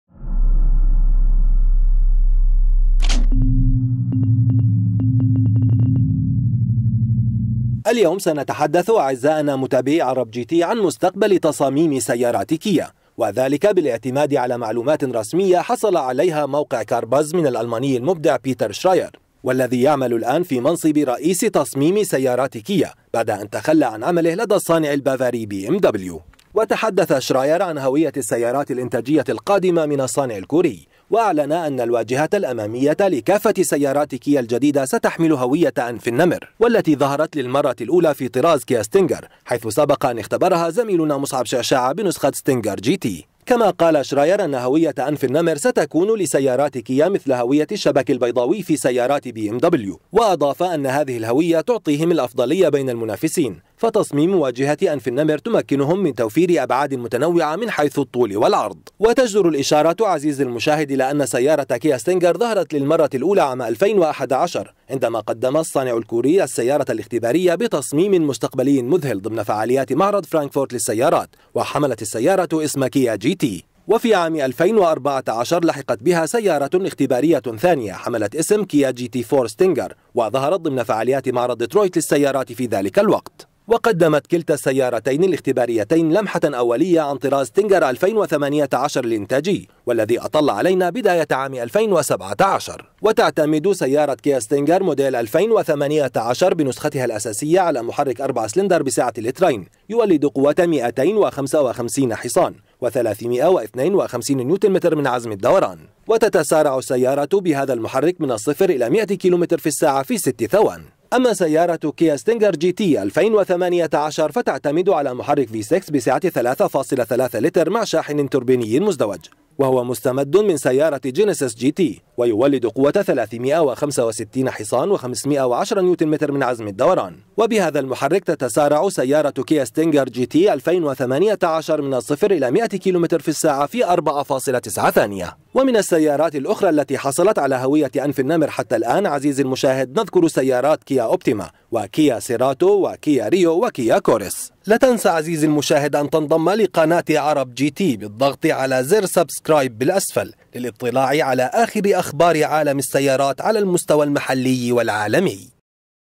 مستقبل تصميم سيارات كيا الإنتاجية القادمه
يتر شراير تحدث عن هوية السيارات الإنتاجية القادمة من الصانع الكوري كيا موتورز ، وأعلن أن الواجهة الأمامية لكافة السيارات الجديدة التي ستصنعها كيا ستحمل هوية أنف النمر ، التي بدأت مع طراز كيا ستينجر
مرسيدس مايباخ التيمت لكجري الـ SUV الفارهة الجديدة تكشف عن نفسها
بي ام دبليو M2 كومبيتيشن 2019 حصلت على هذه التحديثات وعلى قدر إضافي من القوة